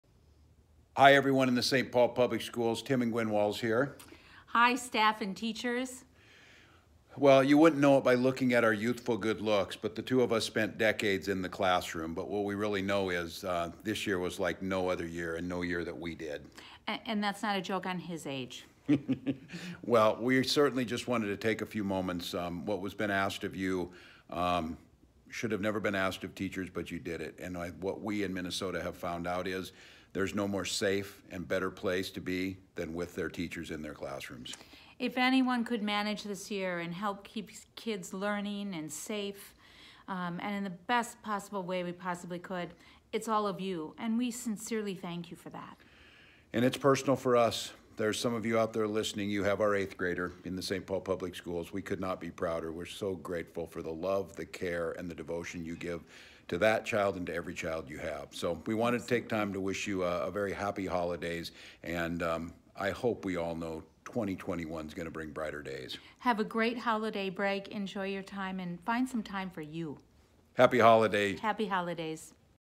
Thank you from Governor and First Lady Walz
Minnesota Governor Tim Walz and First Lady Gwen Walz thank Saint Paul Public Schools staff for their tireless efforts and look forward to a brighter new year.